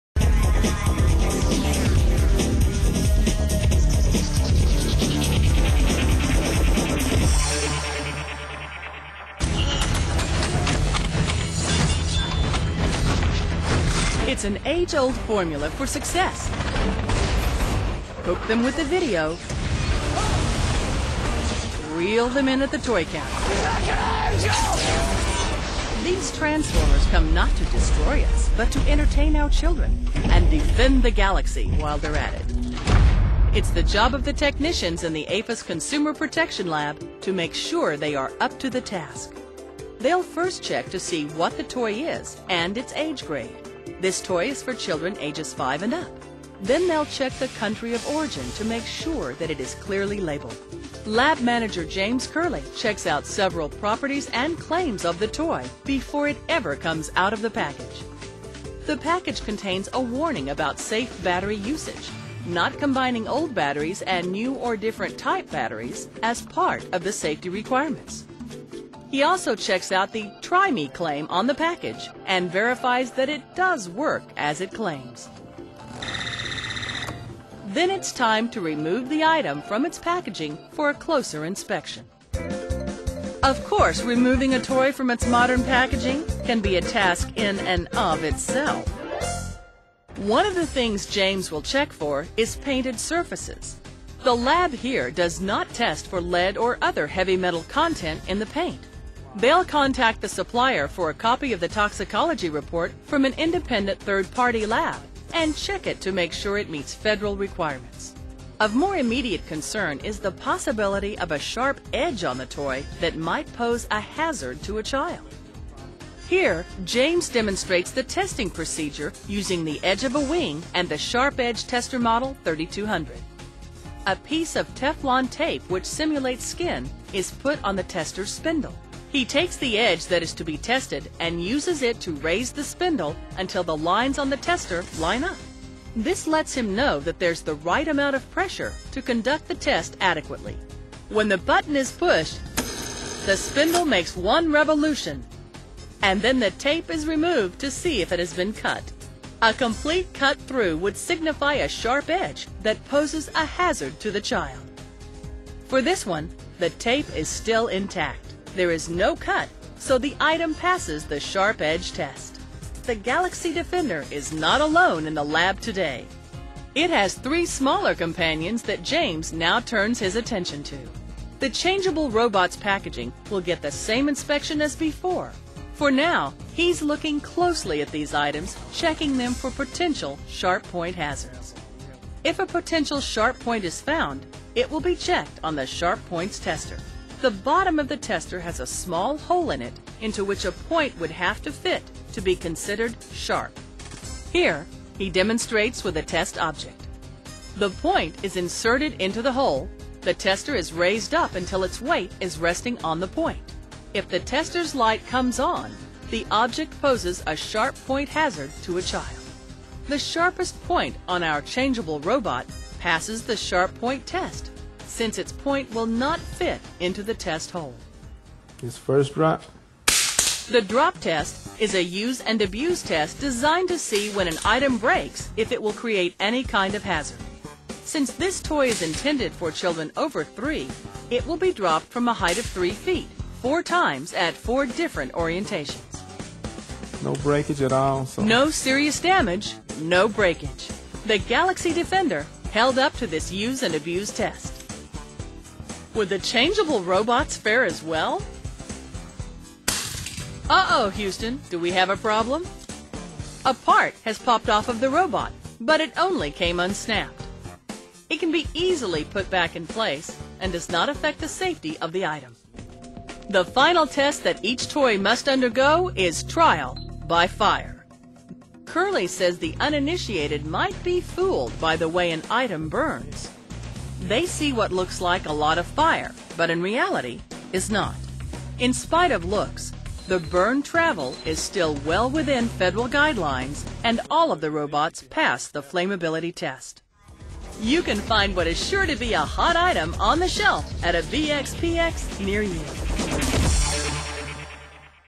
Toy robot product safety testing
Product testing laboratory evaluates the safety of the HappyWell Galaxy Defender Valkyrie.